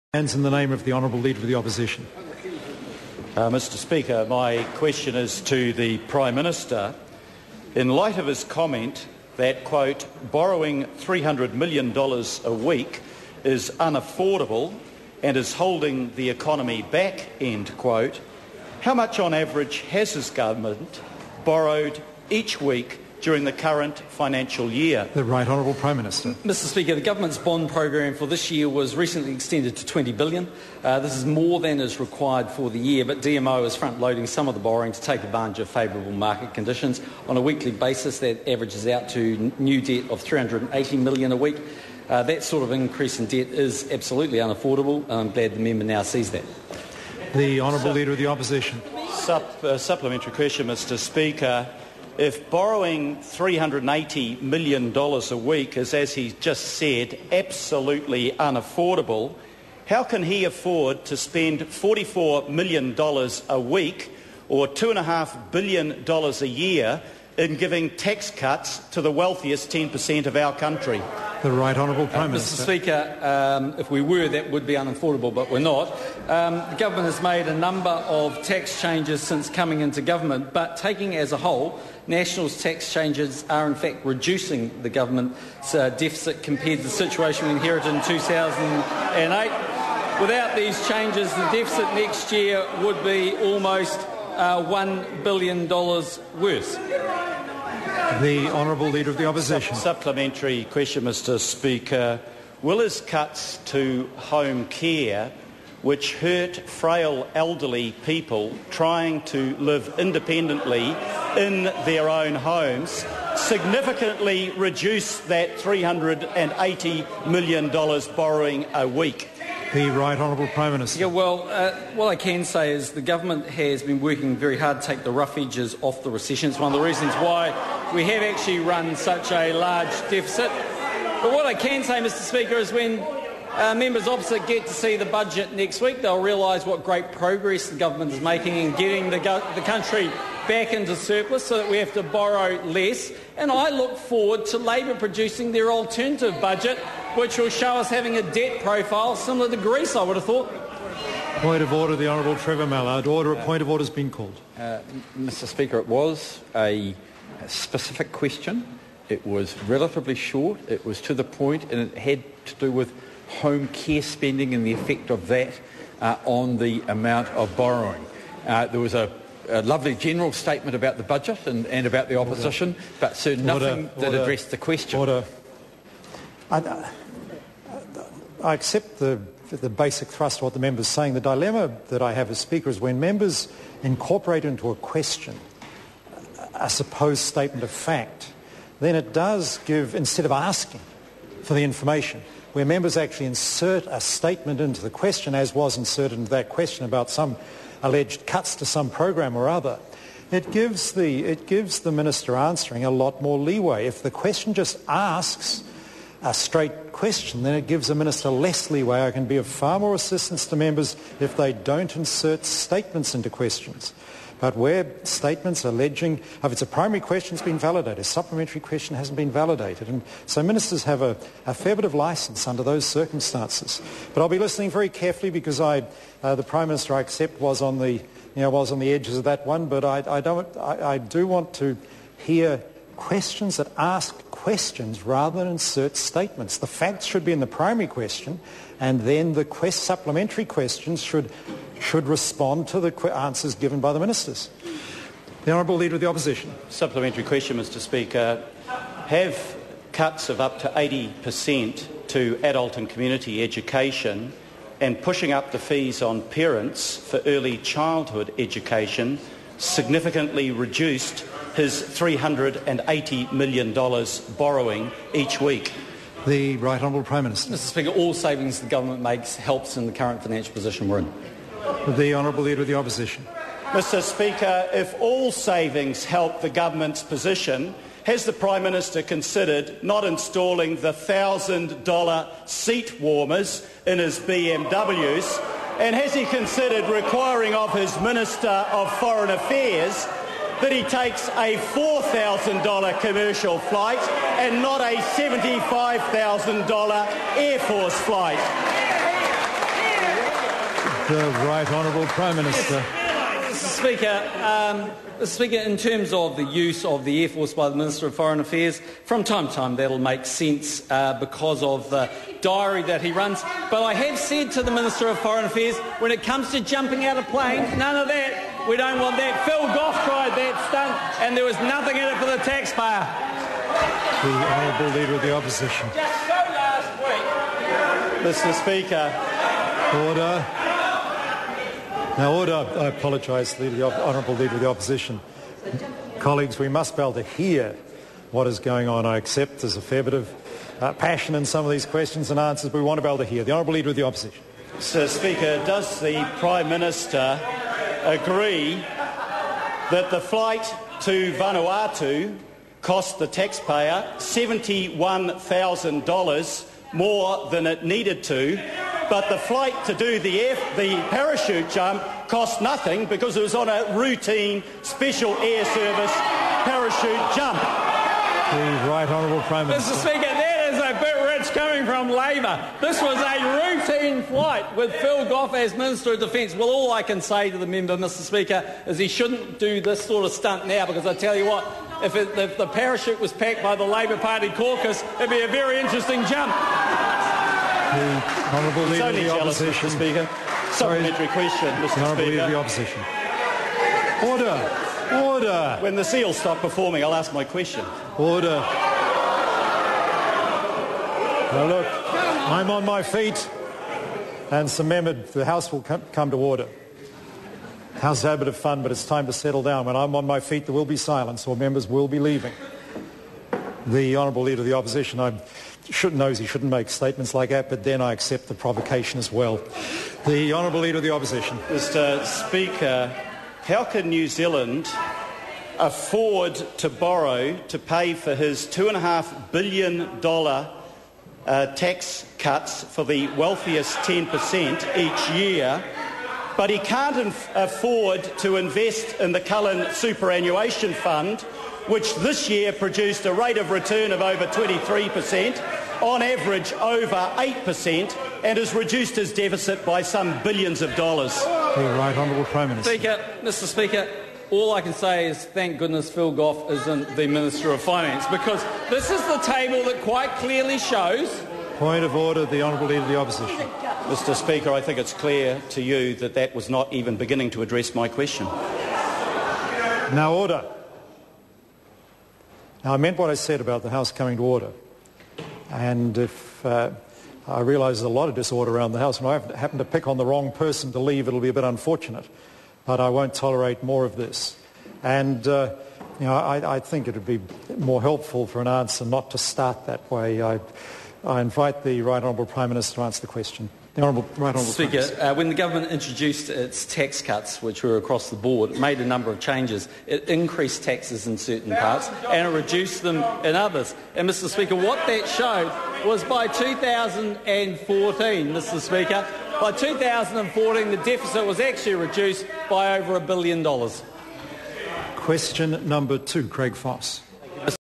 10.5.11 - Question 1: Hon Phil Goff to the Prime Minister
In light of his comment that "borrowing $300 million a week is unaffordable and is holding the economy back," how much on average has his Government borrowed each week during the current financial year?